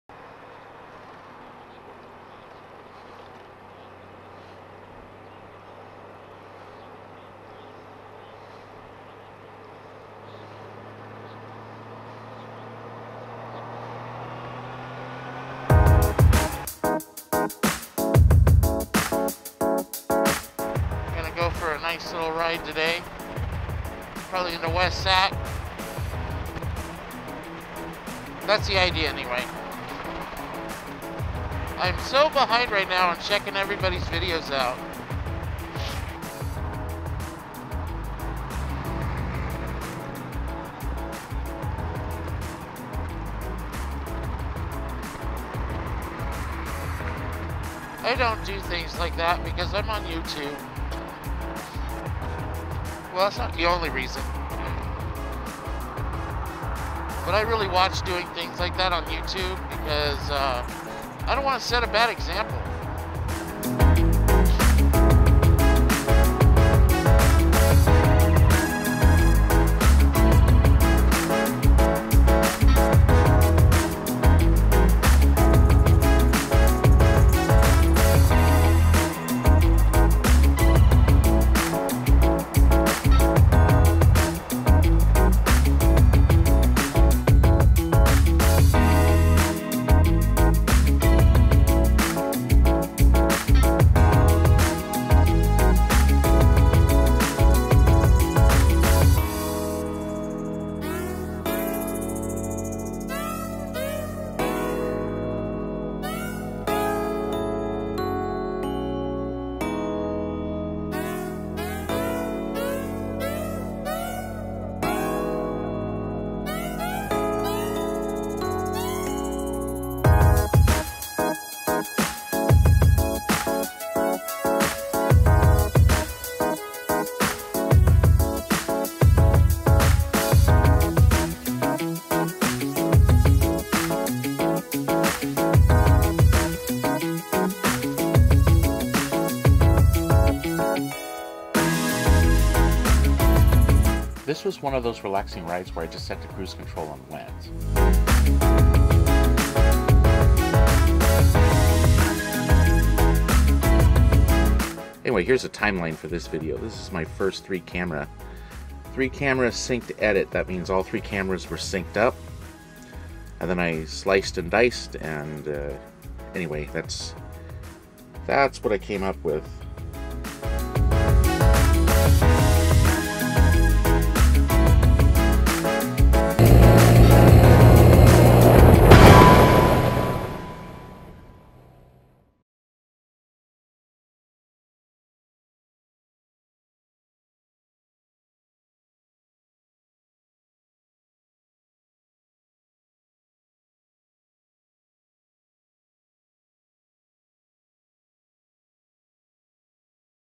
just a ride; Urban Days Epidose 32; first time doing a three cam edit on Hit Film Express
Sometimes you just want to take a little ride;

Also the first time I have done an edit in hitfilm, syncing three cameras.  it crashed a couple of times.  Had to export this one three times to get everything right.  Hit film is less forgiving than FCX when it comes to sound.

Cameras
Zoom q4
Gopro Hero
Lightdow 4000
Nikon 3200
Nikon 3300